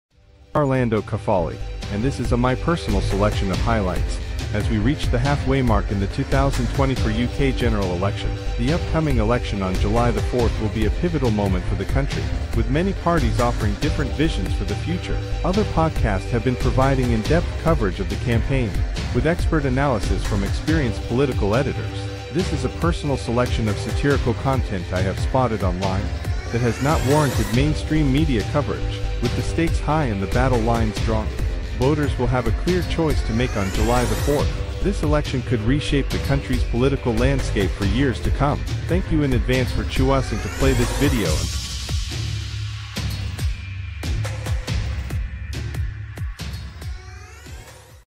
Parlando Kefali Podcast ~ A Mankmistro -Mithistomia Production (Attropiation Edit)
I am Parlando Kefali, and this is a my personal selection of highlights as we reach the half way mark in the 2024 UK general election.

The upcoming election on July 4th will be a pivotal moment for the country, with many parties offering different visions for the future.

Other podcasts have been providing in-depth coverage of the campaign, with expert analysis from experienced political editors.

This is a personal selection of satirical content I have spotted on line that has not warranted mainstream media coverage.

With the stakes high and the battle lines drawn, voters will have a clear choice to make on July 4th. This election could reshape the country's political landscape for years to come.

Thank you in advance for chooisng to play this video and for your time.

This video features some words that may not feature in any dictionary and with which you may be unfamiliar.  You are most welcome to do web searches on those words

Background

This video references mithistomia - including Parlando Kefali, the Volksempfangerfon App, the toolmakers (ice pick reference to Trotsky), the Non Repudation Act  and the C5.

The last was inspired after finding that the middle name of a prominent UK politician was "Rodney".

That was also the name of a fictinal character ina popular long-running television series in the UK which featured two brothhers (Derek and Rodney) who used a yellow three-wheeled vehicle embalzoned the "Trotters Independent Trading".

Fast forward to the 2020s - and a revamped design named the "C5+" after the three wheeled electric transport vehicle designed by Sir Clive Sinclair - with reference also to Cairncross, Blunt, Burgess, McClean and Philby who were known as the "Cambridge Ring" - and the suspicion that other Communists continue to subvert the UK.

The 3 wheel design is adept at navigating the potholes on UK road infrastructure and require 25% less tires than conventioal 4 wheeled vehicles.  If "Net Zero" is not overturned - then may be possible for the "4th wheel"  be traded as a carbon credit and help fund the spending plans needed to revalise the UK economy ;-)